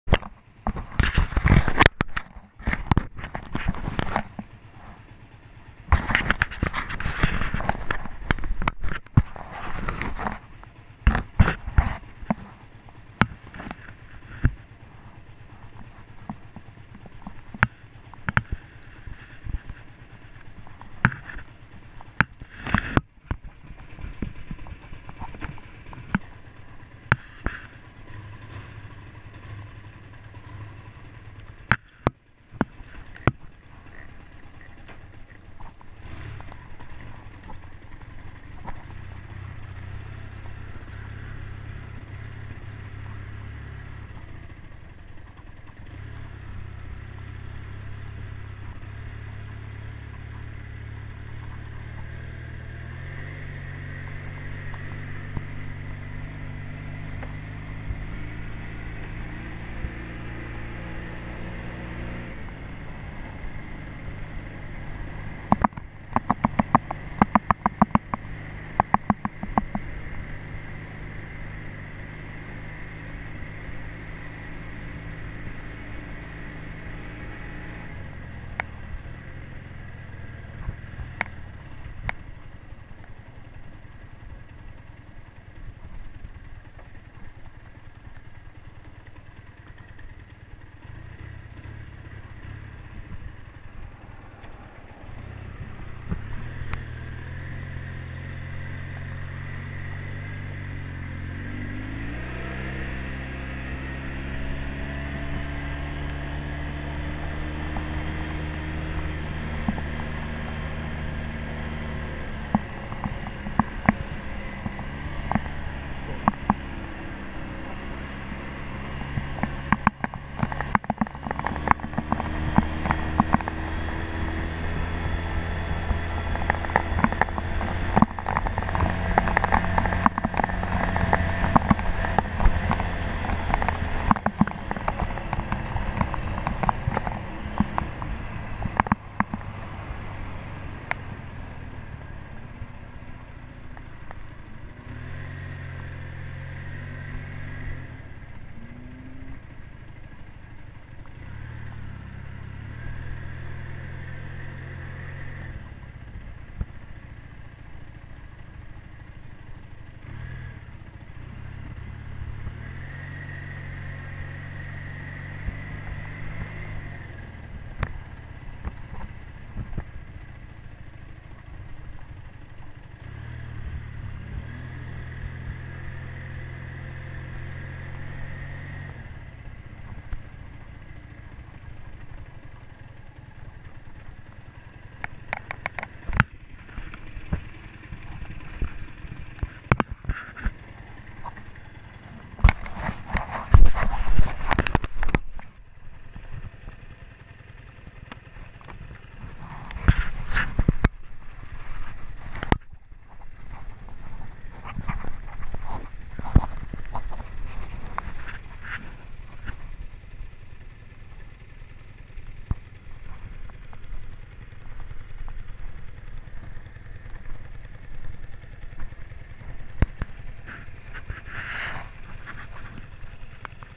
1978 Honda CB400a Hondamatic Motorcycle
Quick ride on my new(vintage) 1978 Honda CB400a Hondamatic motorcycle.